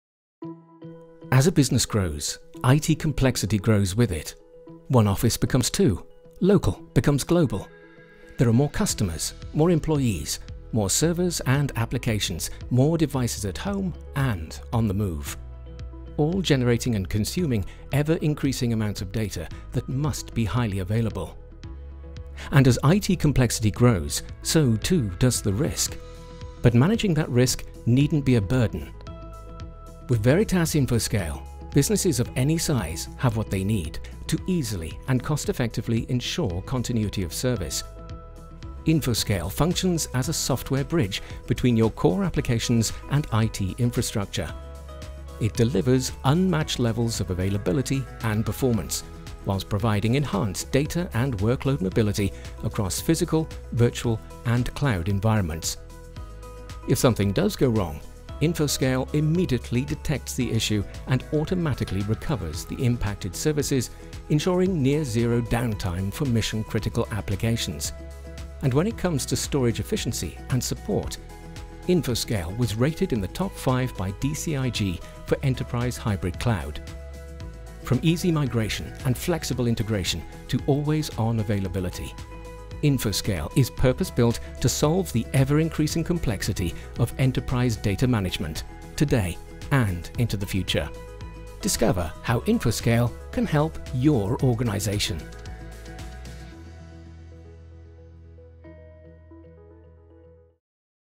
Conquer IT Complexity and Achieve Always-On Availability with Veritas InfoScale™
See how Veritas InfoScale provides unified resiliency and IT business continuity across all platforms and operating systems, no matter where they live.